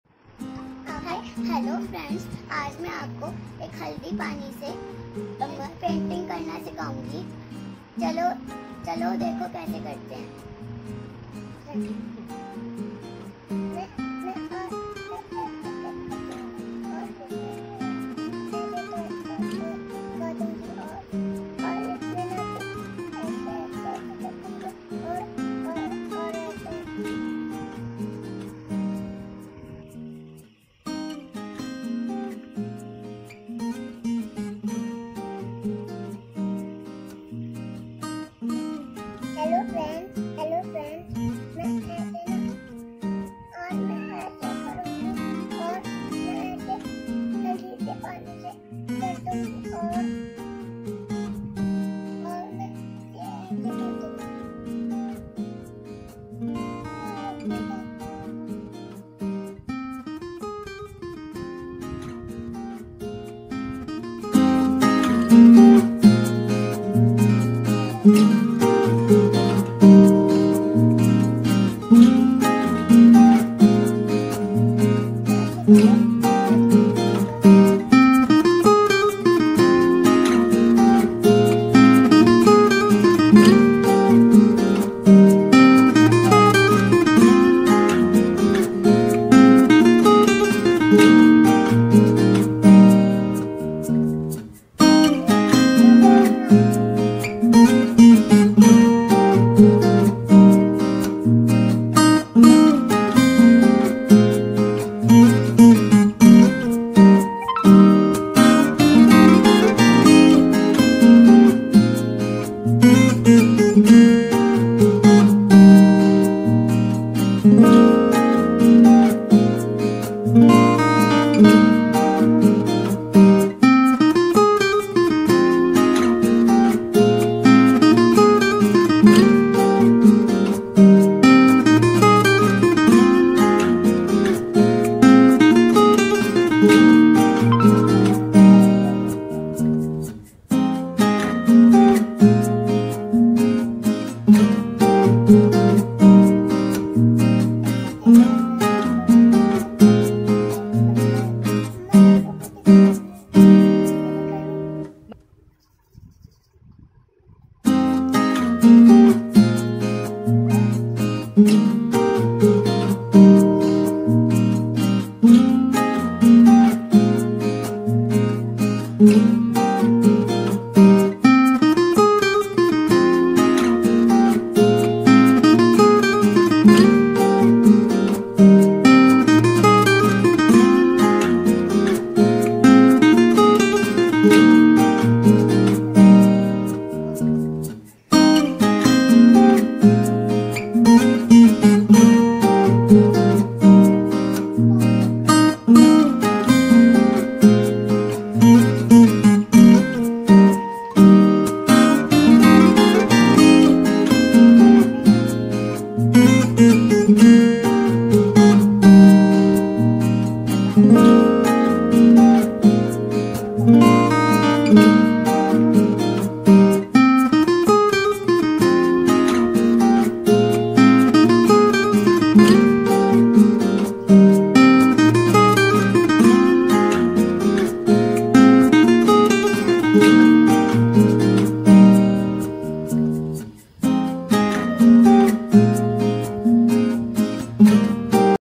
Eco-Friendly Art With Haldi Water For Kids From Sibling Kids
Hello Friends...here is video about coloring idea with eco friendly color. This activity can be a good option for two kids as well. Hope you like the video. Thank You.